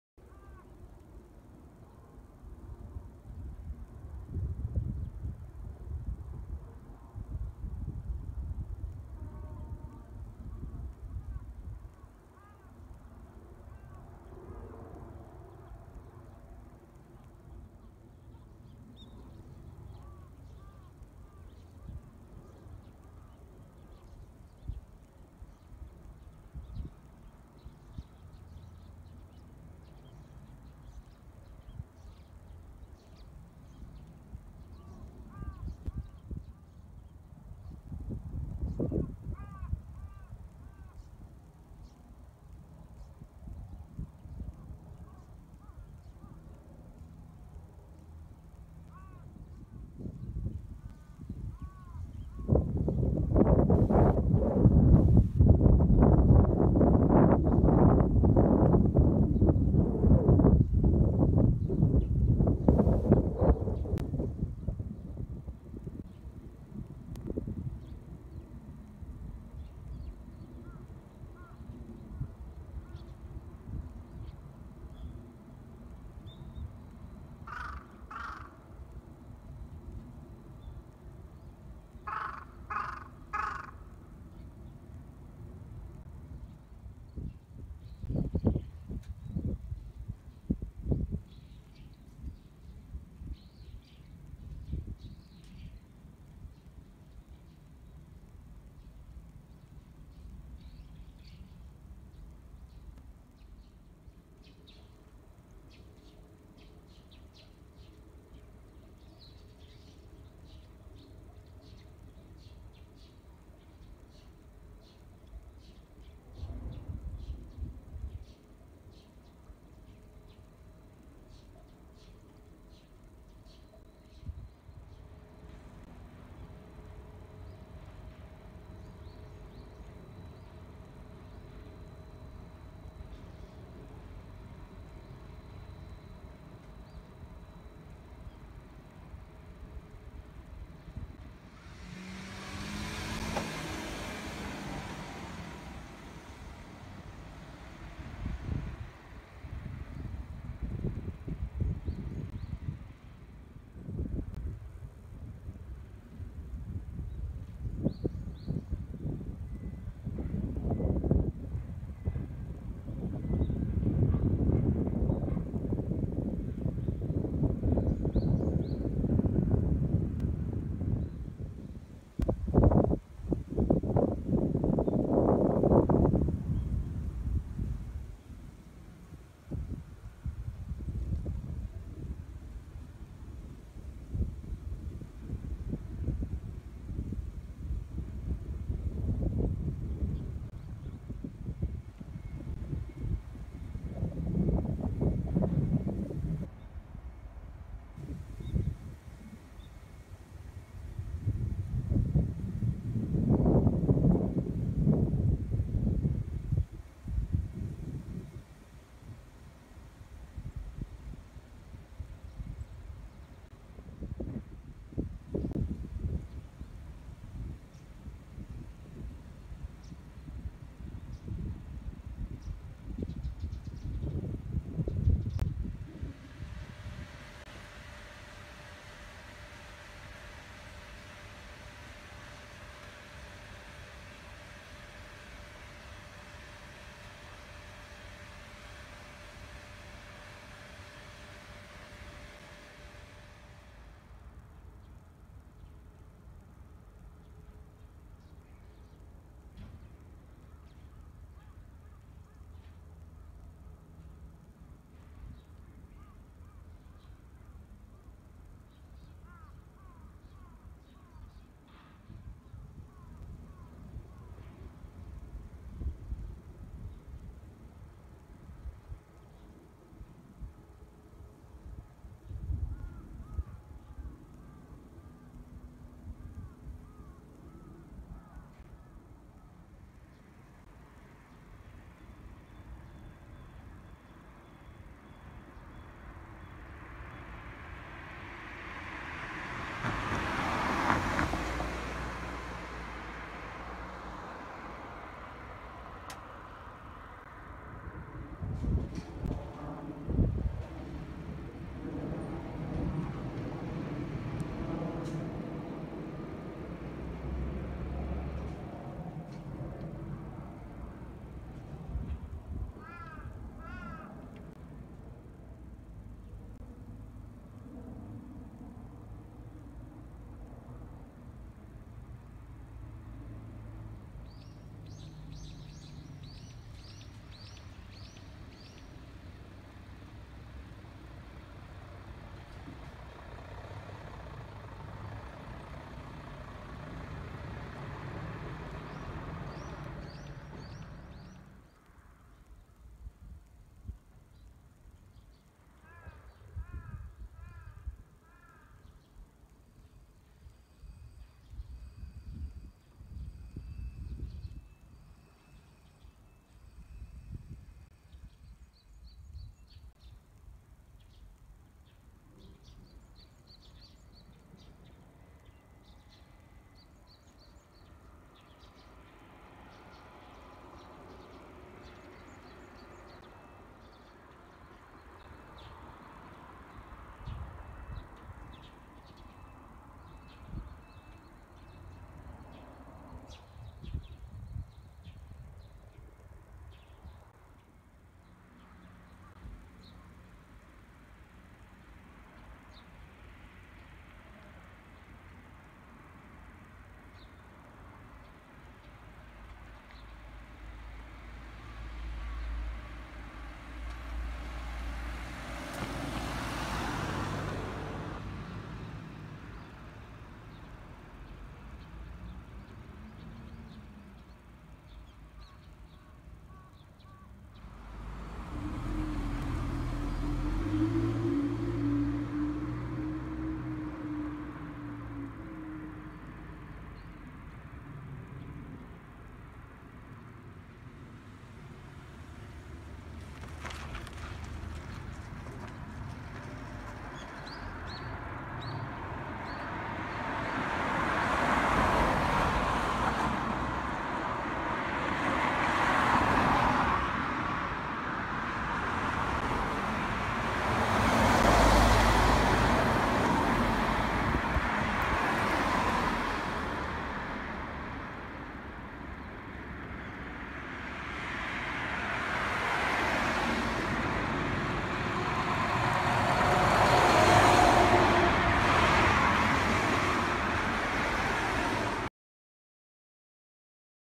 Япония. Ибараки кен. Провинция.
Это точно Ибараки )))

Райончик, где мирно соседствуют заводы и заводики с жителями селения.

Просто так - прогулка ))) Понедельник, примерно между 13-00 и 14-00